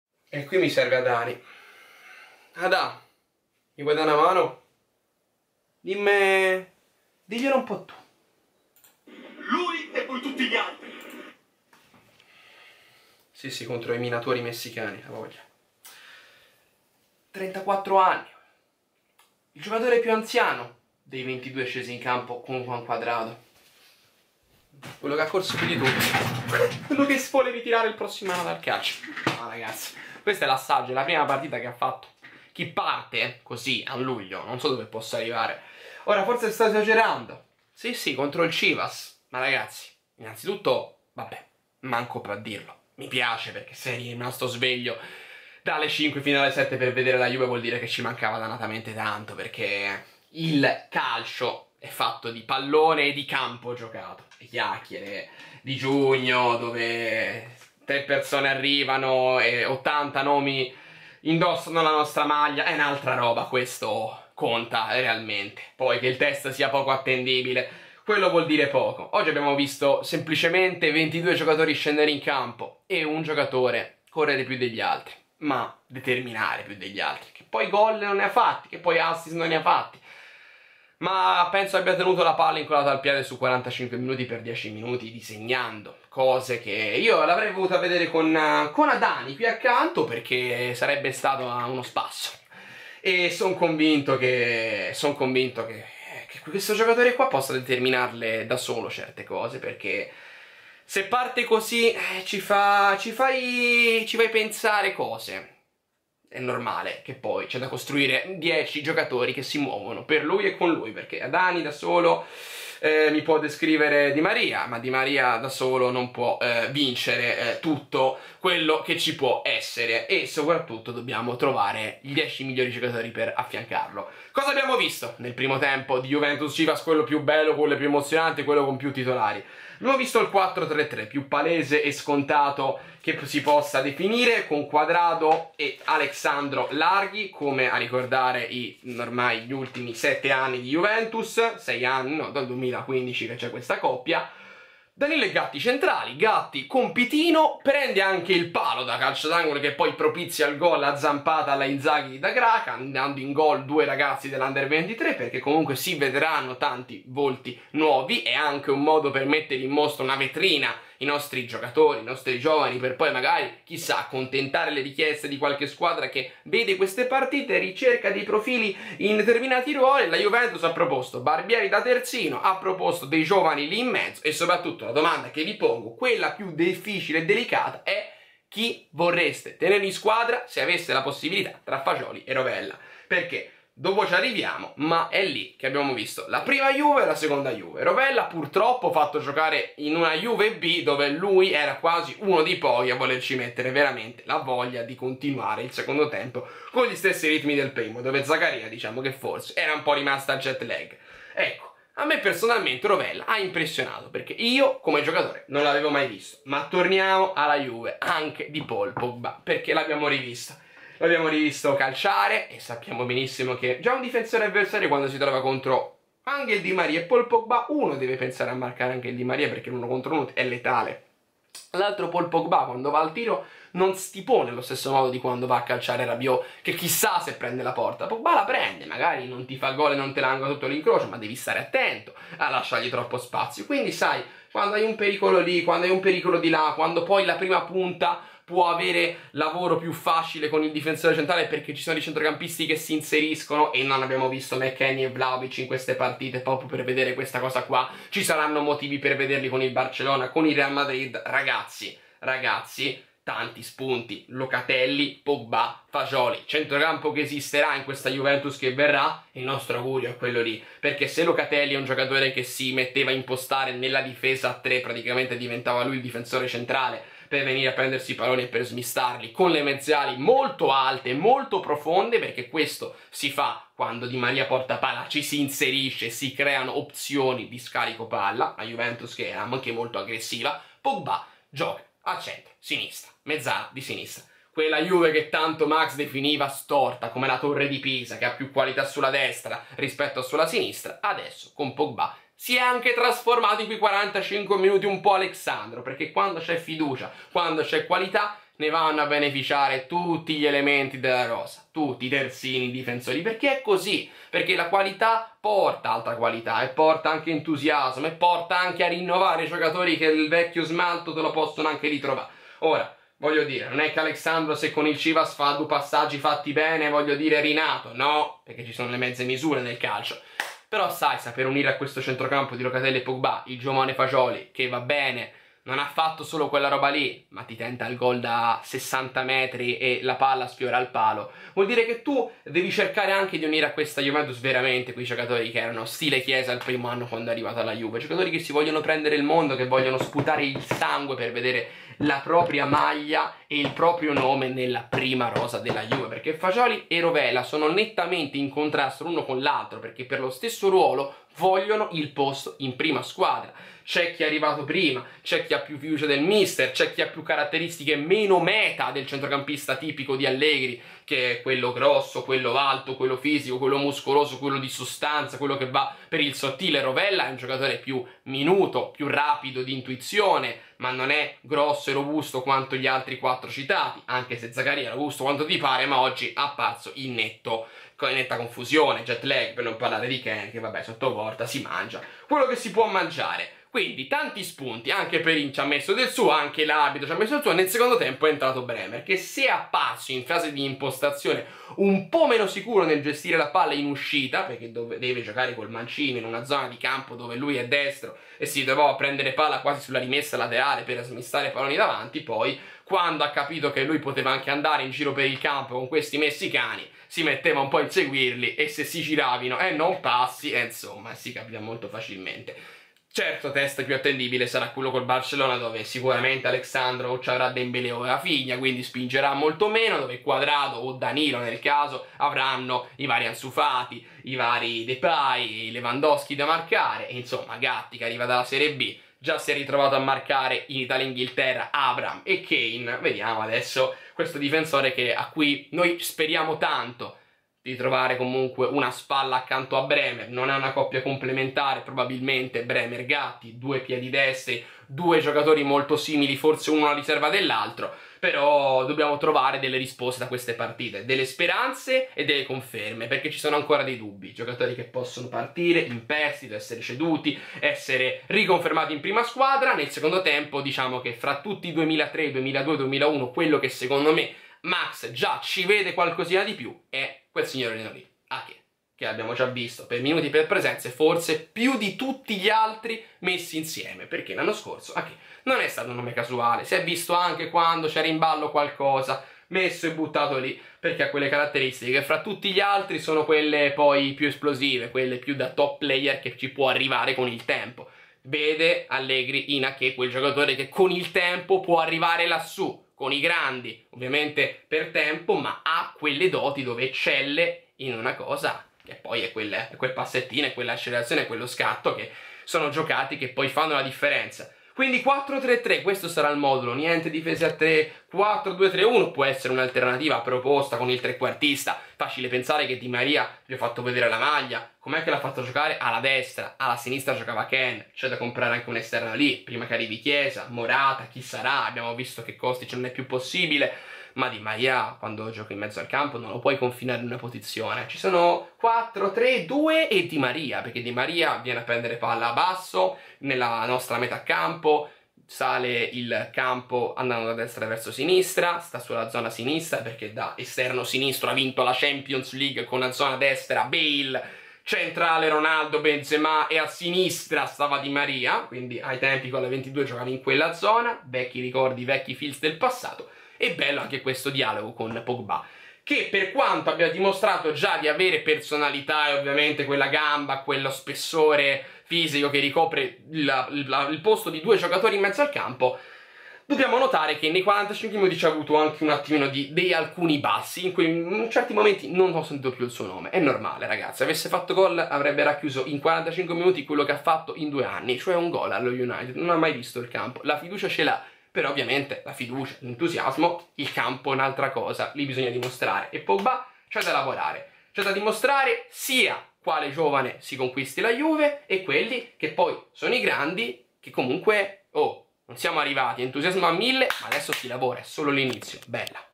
MI SONO INNAMORATO.
Juventus Chivas 2-0 (gol da graca, compagnon)

Bianconeri di Massimiliano Allegri pronti a scendere in campo per la prima volta nella nuova stagione 2022-23, seppur in un amichevole internazionale, precisamente negli States
Ultima gara ufficiale distante ormai 2 mesi, in quel lontano 21 Maggio contro la Fiorentina in Serie A
I giovani Under23 che partono per gli Stati Uniti con la Juventus: Soulé, Aké, Cudrig, Da Graca, Garofani, Barbieri, Compagnon e Fagioli

Soccer Champions Tour che continuerà mercoledi con il Barcellona e Domenica con il Real Madrid